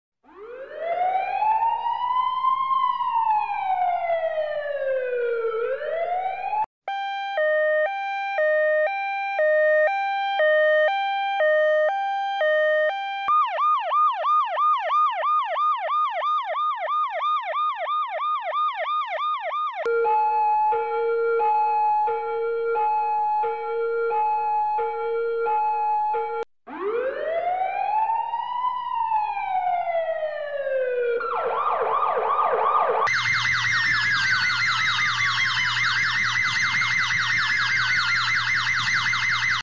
USA vs Spain Police, Ambulance, Fire Truck Siren Horn Sound Variations in 40 Seconds
This is new video USA vs Spain Police, Ambulance, Fire Truck Siren Horn Sound Variations in 40 Seconds i created for you!